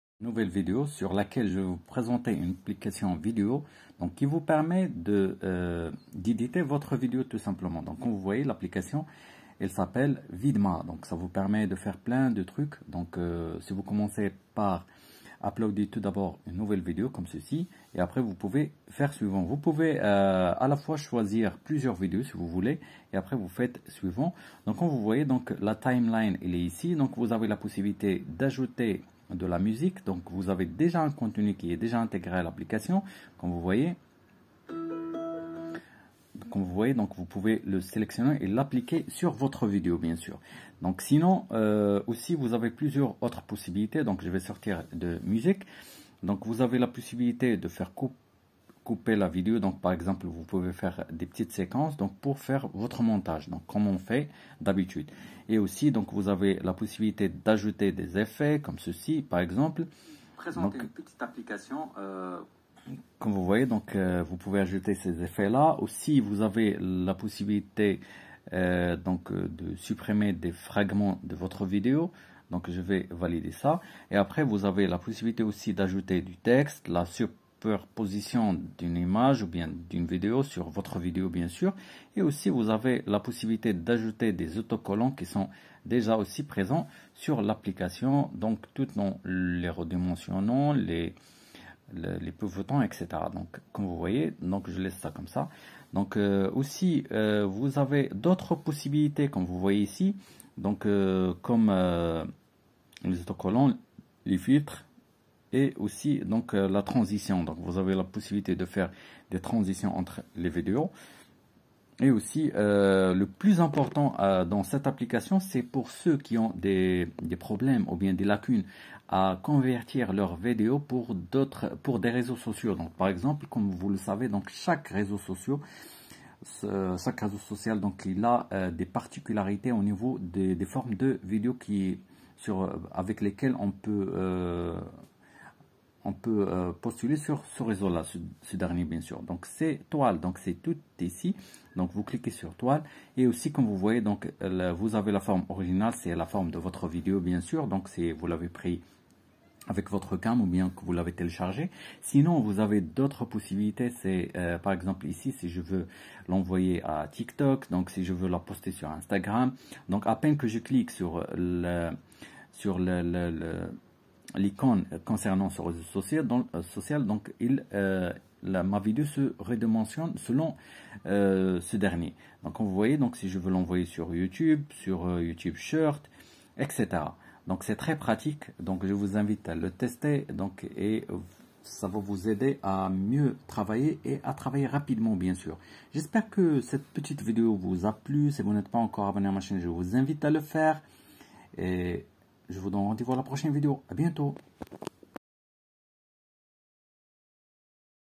montage vidéo sur Android avec l'application vidmate
sur cette vidéo je vous présenter une application sous Android qui vous permet de faire un montage professionnel ainsi que aiguiller votre vidéo sur différentes réseaux sociaux avec les dimensions voulues